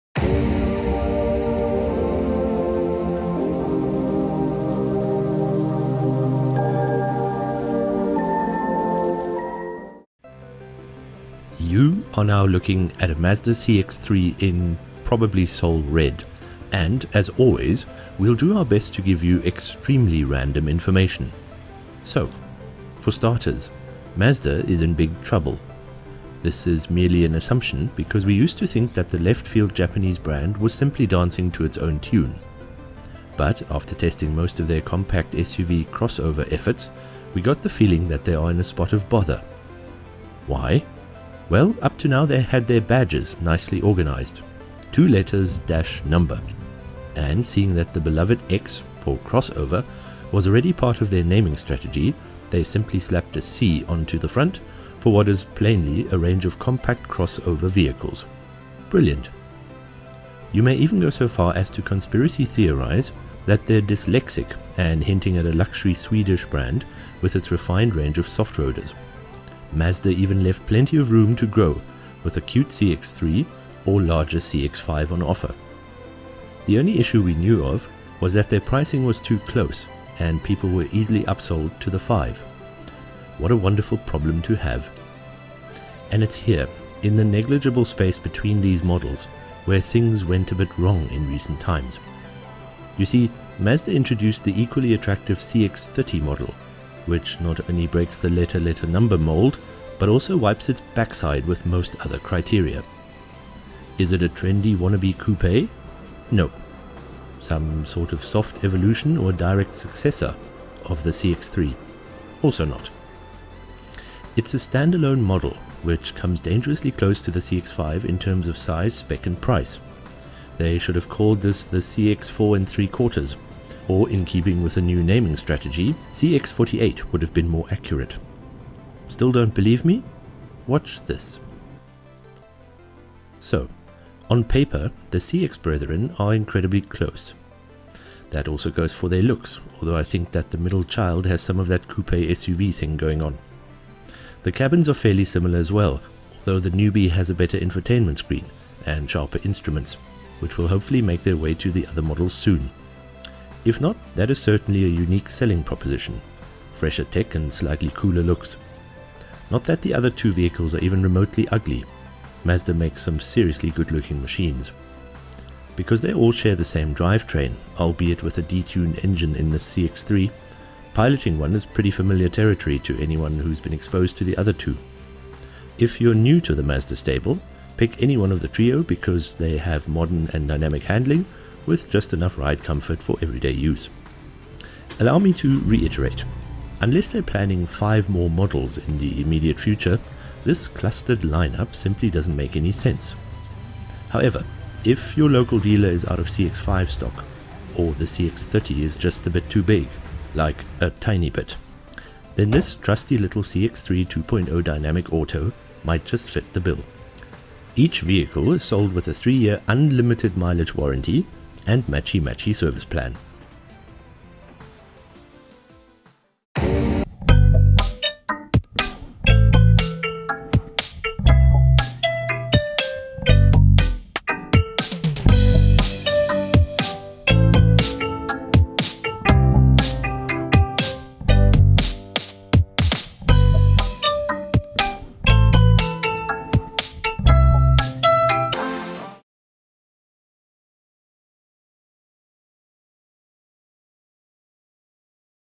Video Review: 2022 Mazda CX-3... and CX-5. But mostly, CX-30 😆
GalimotoMedia Video Review of a MY22 Mazda CX3 2L Dynamic Auto(matic) but also its bigger and jolly confusing siblings!